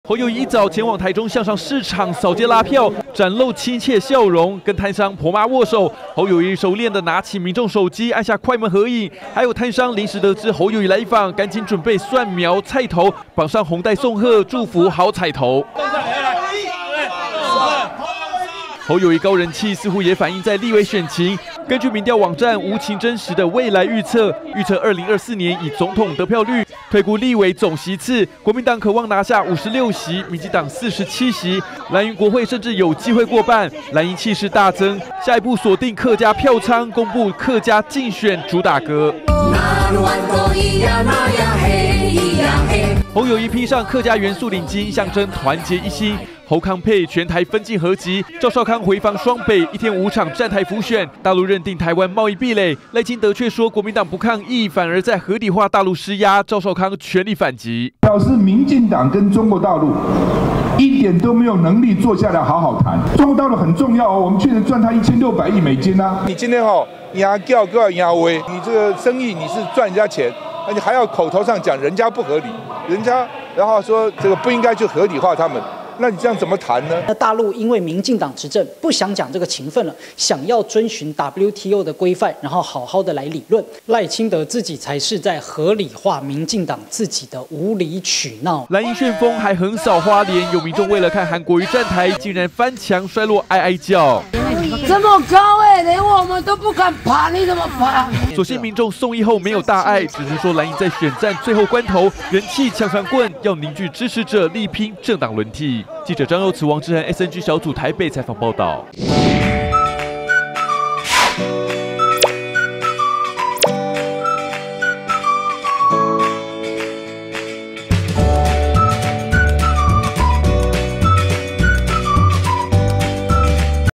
【每日必看】侯康配"分進合擊"人氣旺 藍營力拚國會席次過半 20231217
侯康配把握倒數周末，兩人分進合擊，趙少康今天(17)回防雙北，怒轟民進黨沒法跟大陸溝通，尤其大陸認定台灣貿易壁壘，賴清德竟然說"藍營在合理化對岸施壓，趙少康反問"這樣該怎麼溝通"，而侯友宜到台中市場拉票，受到婆媽歡迎，藍營氣勢大增，網路預測分析，藍營選情整體看好,立委有望拚"國會過半"。